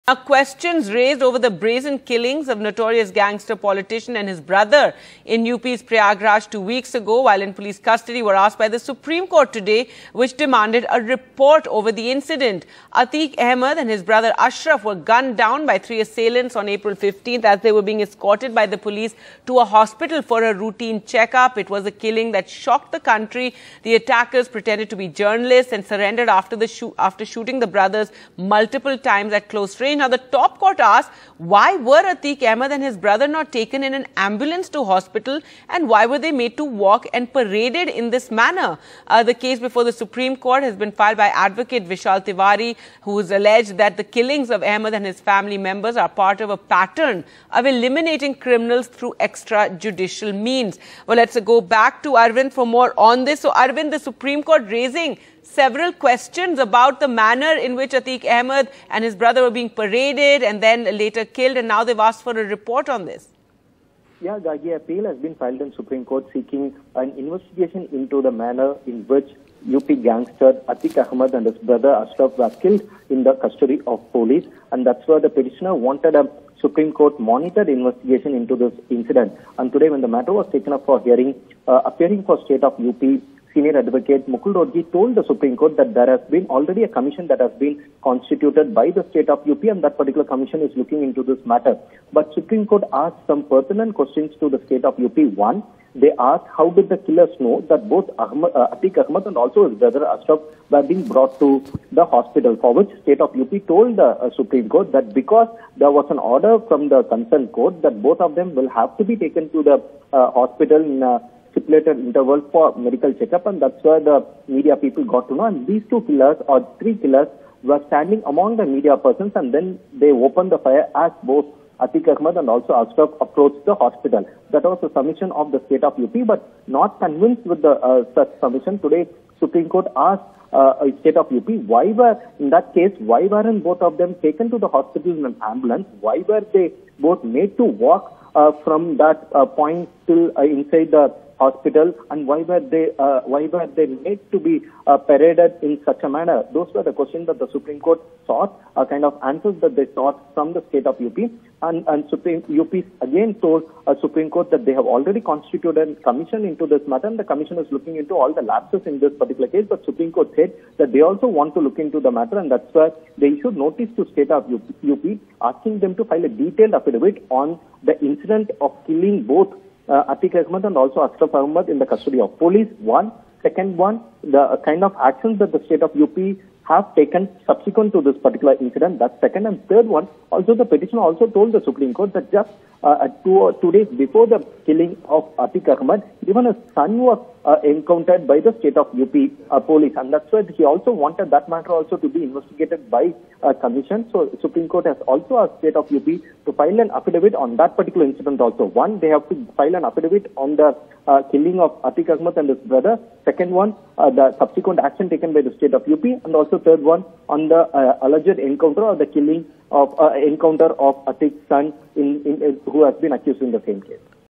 "How Did (Killers) Know?" Supreme Court Questions For UP On Gangster Murder
Atiq Ahmed Murder Case: Questions raised over the brazen killings of a notorious gangster-politician and his brother in Uttar Pradesh's Prayagraj two weeks ago while in police custody were asked by the Supreme Court on Friday, which demanded a report over the incident.